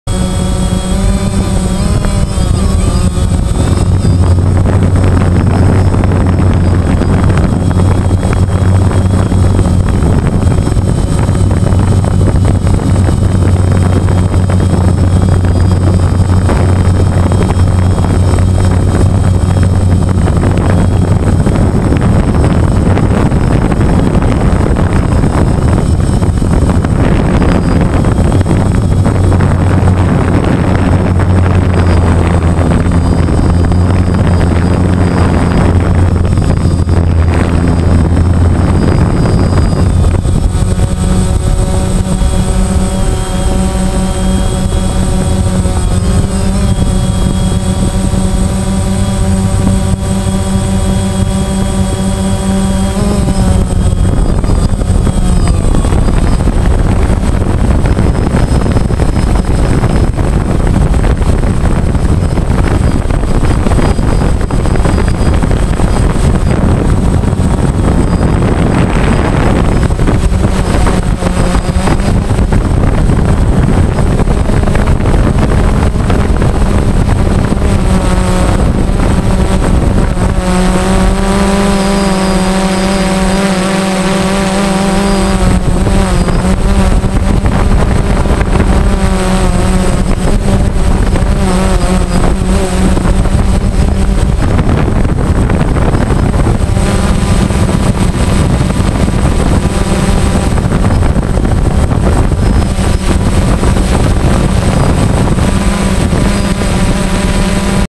Mount Juliet, Thomastown from the Sky
Short Video of Mount Juliet, Thomastown from the Sky. Captured with a DJI Phantom, GoPro Hero 3 and Fat Shark Goggles.